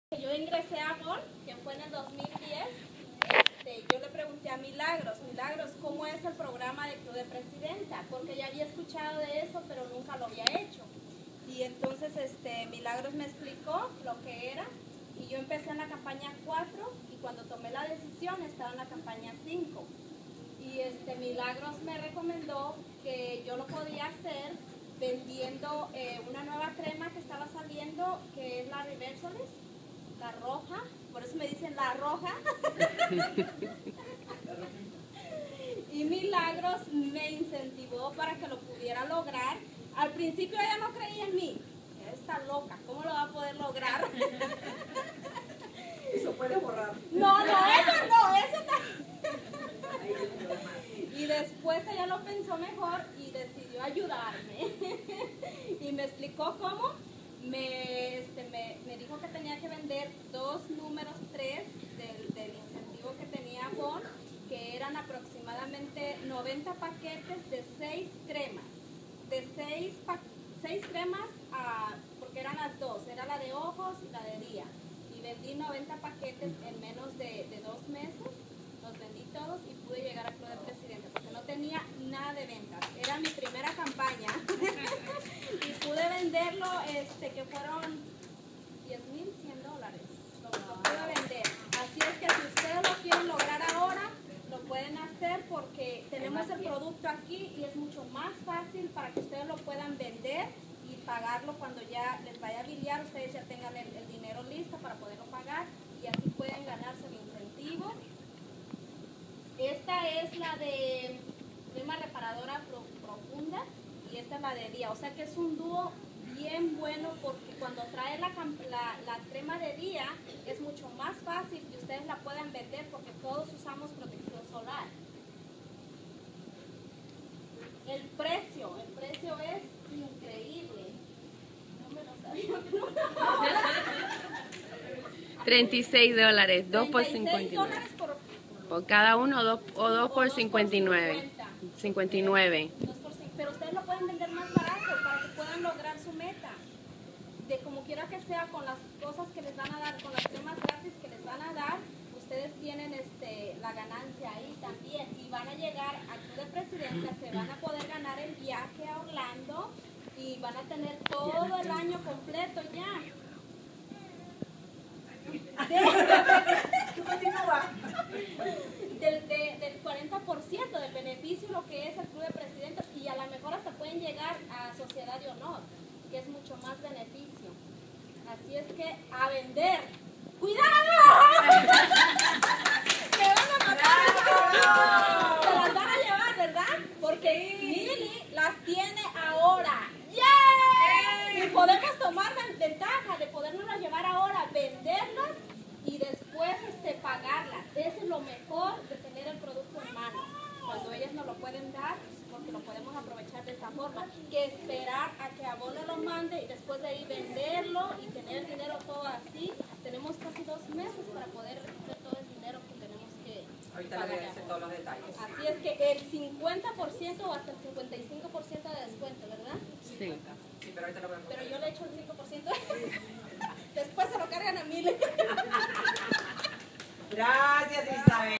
SEUL Isabel comparte Como vendio $10,100 en 1 mes para llegar a Club de Presidente
Testimonio de trasar metas y Como lograrlas. Isabel comparte su estrategia y Como ganar con Ventas. Comienze hoy su negocio de Avon con solo $10.